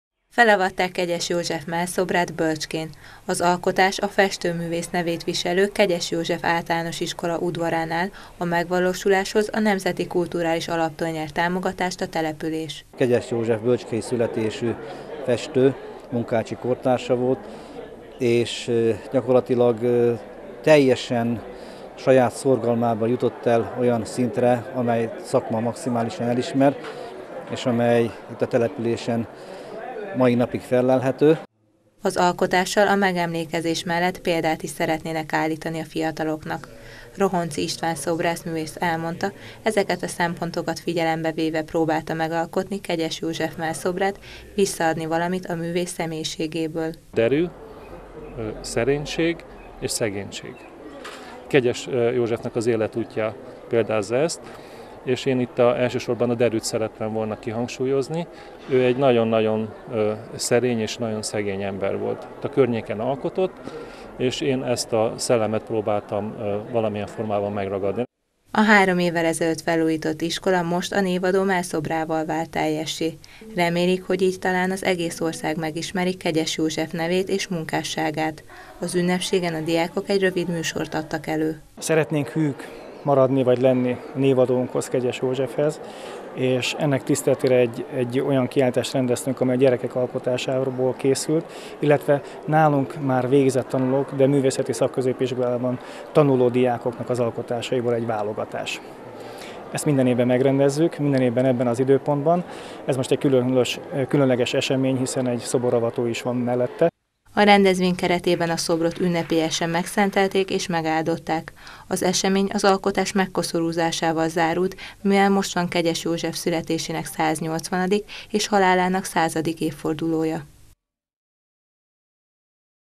Kegyes József szobrot avattak Bölcskén
Kegyes József szobrot avattak Bölcskén, a nevét viselő helyi általános iskola udvarán. A mellszobor állításához a Nemzeti Kulturális alaptól nyert támogatást a település.